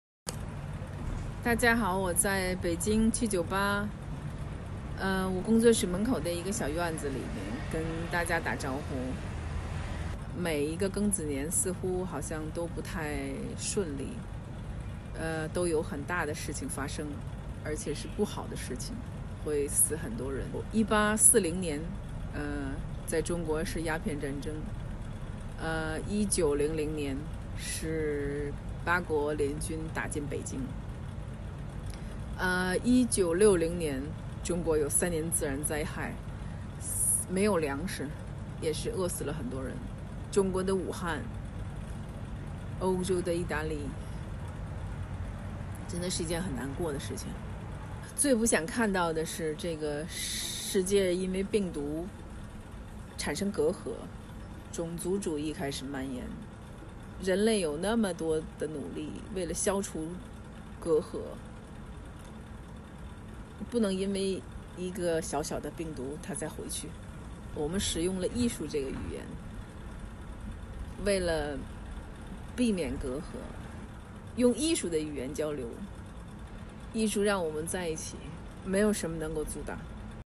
Peng Yu's message from Sun Yuan & Peng Yu's studio in Beijing, China
ContinuaArtistsWithYou : Peng Yu

A beautiful message and video shared with us by Peng Yu, part of the duo SunYuanPengYu , from their studio in Beijing !
“This is a really devastating time, the last thing we need to see is this virus dividing us apart and creating more racism around the world. Human have made so many efforts in the history to eliminate the division we shouldn’t step backwards because of one virus. That’s why we use art as our common language to avoid any separation between us as long as we stand together with art and love nothing can’t be conquered” Peng Yu.
To share mutual support with you ContinuaArtistsWithYou brings Continua’s artists to you with unique content from our artists to go through these complicated times altogether!